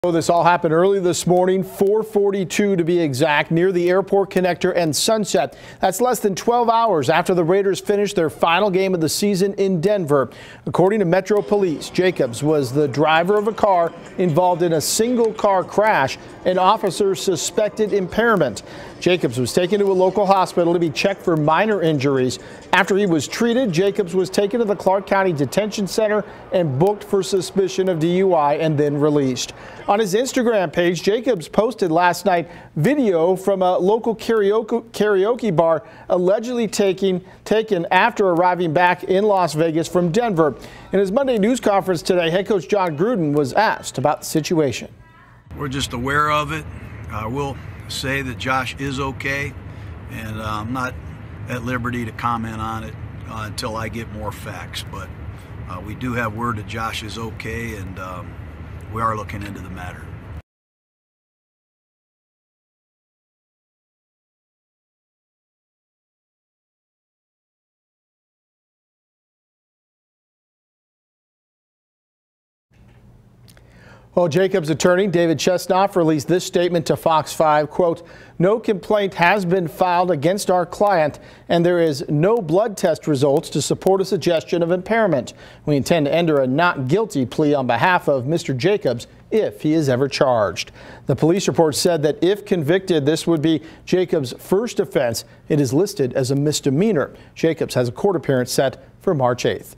Raiders running back Josh Jacobs arrested on DUI charge in Las Vegas
Raiders running back Josh Jacobs was arrested Monday morning on a DUI charge, according to Las Vegas police.